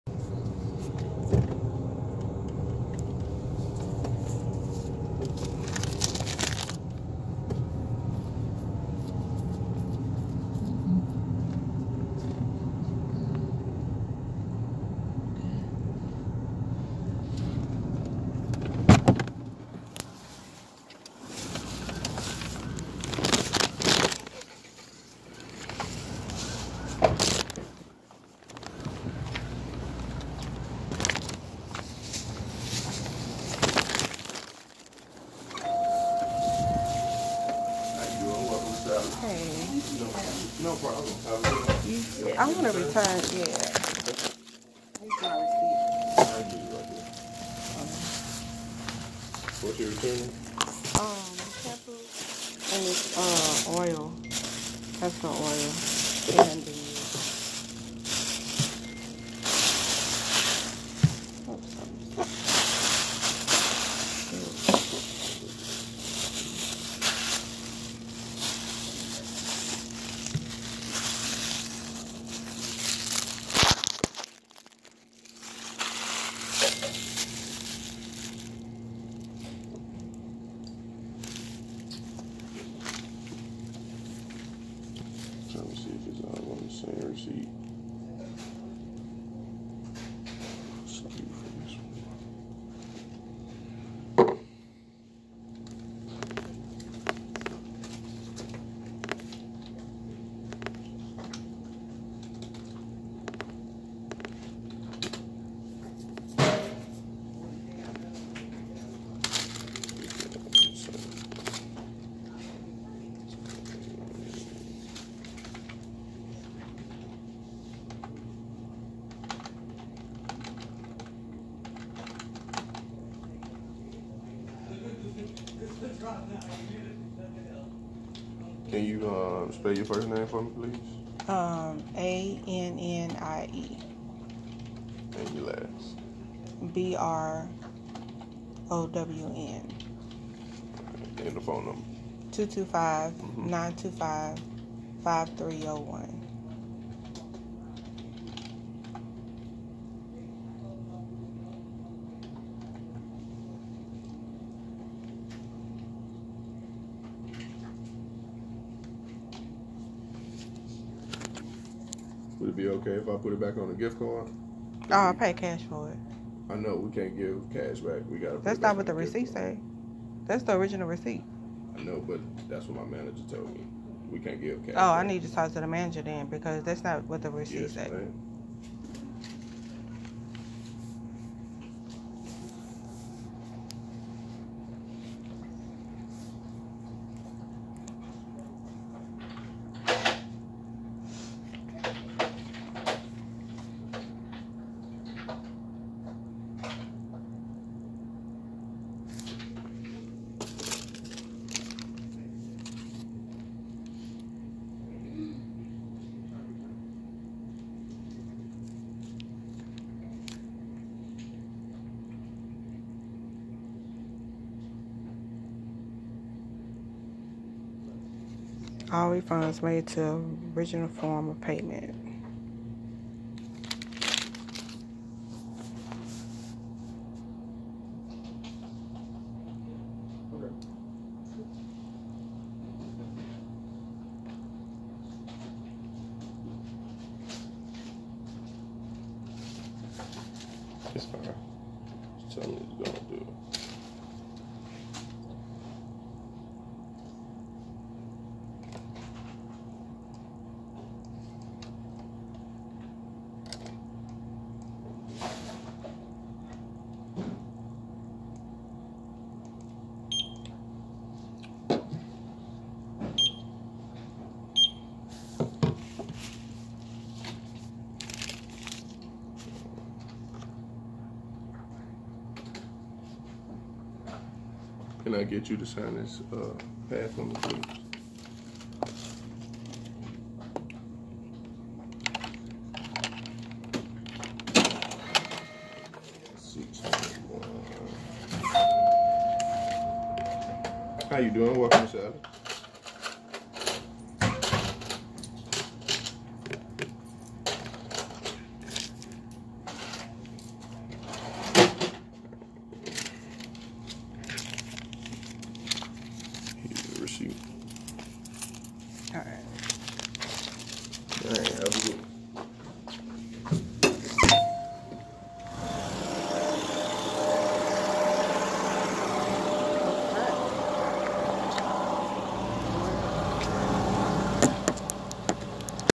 Tuesday's Sally's Return 9/17/19
The return since there was a bit of controversy concerning the products. So I just returned them.